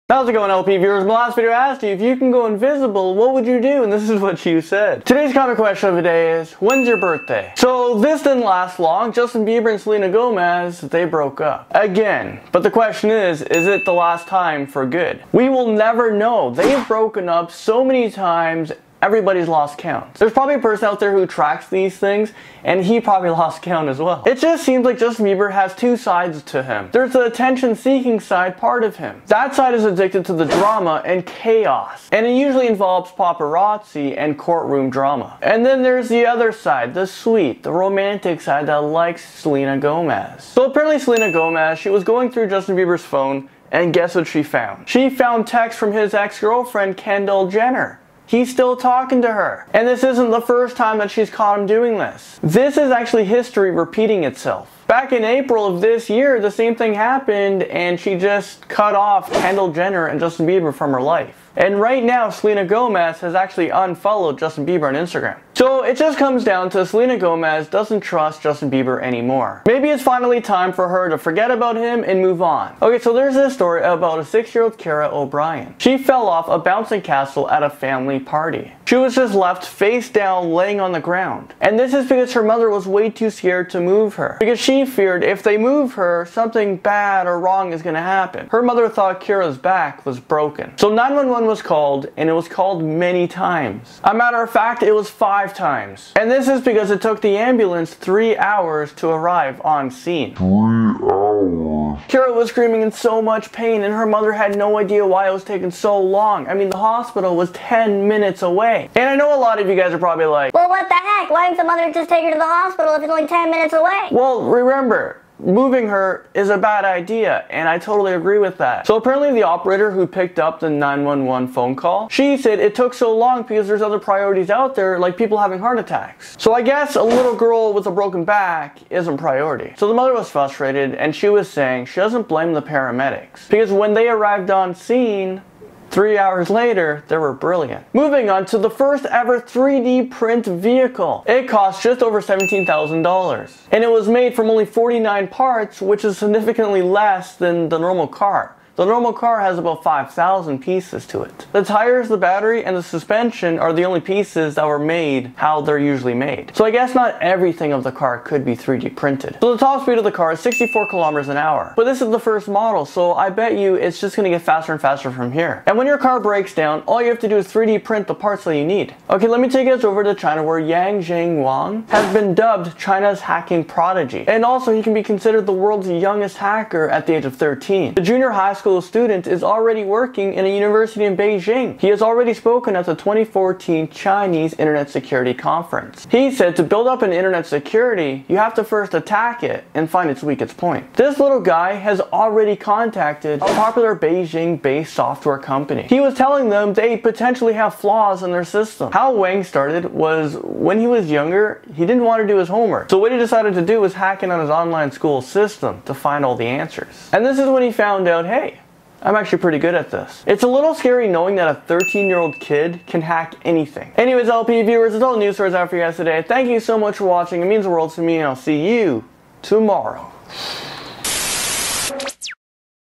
Justin Bieber & Selena Gomez Breaks Up Because Of Kendall Jenner
Justin Bieber and Selena Gomez are through...again. After finding texts to her boyrfriend's ex-girlfriend Kendall Jenner on his phone Selena is done with both of them, perhaps for good?

2408 Lakeshore Blvd W.